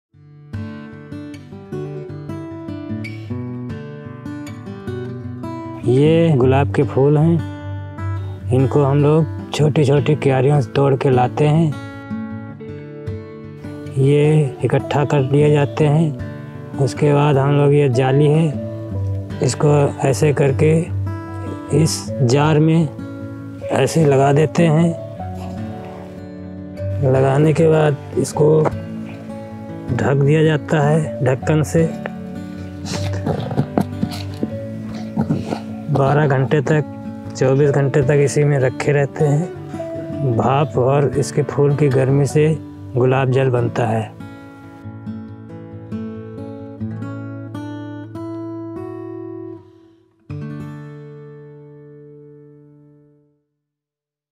Rose Water | Organic Farming | Farm House | Amrut Farm | Farm House in Lucknow
Our process of extracting rose water at Amrut Farm involves lots of patience and diligence.

Firstly, roses plucked from the gardens are wrapped in a net.

These roses are then kept in an airtight jar and left for twelve hours.

The heat from the roses that builds up inside the jar gives birth to 100% pure and natural rose water.

Pay us a visit to get a glimpse of this process at Amrut Farm.
📍Amrut Farm, Nagram, Lucknow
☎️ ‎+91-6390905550